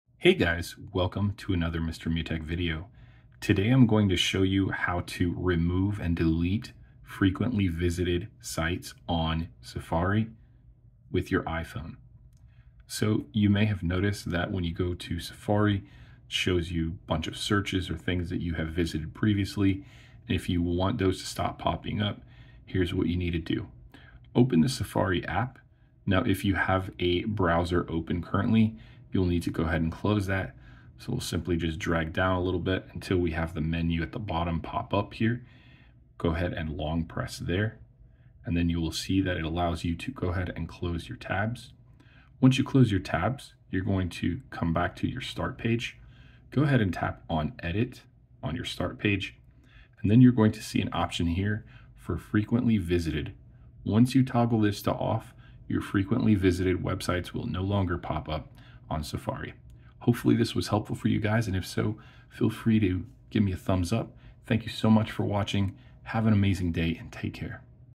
How To Remove Frequently Visited On Safari iPhone
Here is how you can remove frequently visited sites and searches on Safari browser using your iPhone. This will allow you to keep your start page uncluttered and allow you to delete any unwanted visited websites you don't want others to see.

If this was helpful for you please consider leaving a comment.